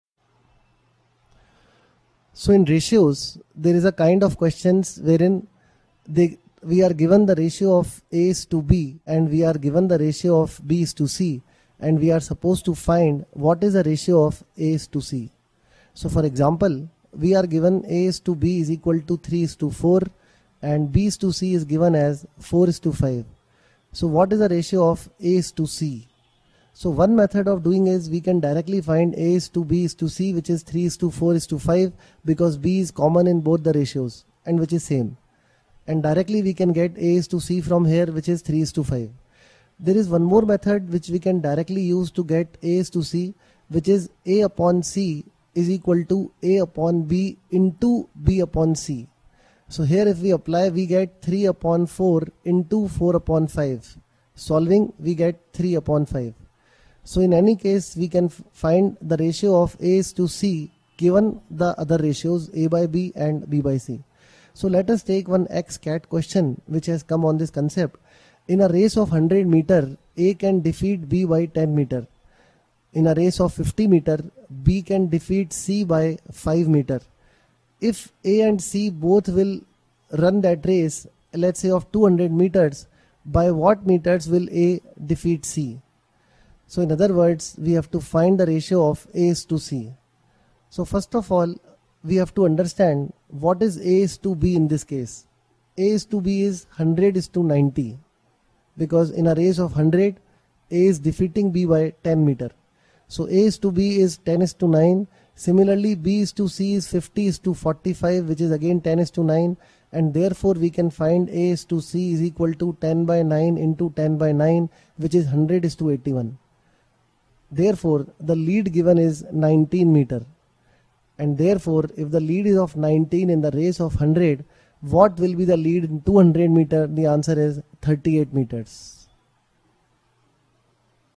Ratio Transitivity Concept
Explains how a ratio can be found out depending on given data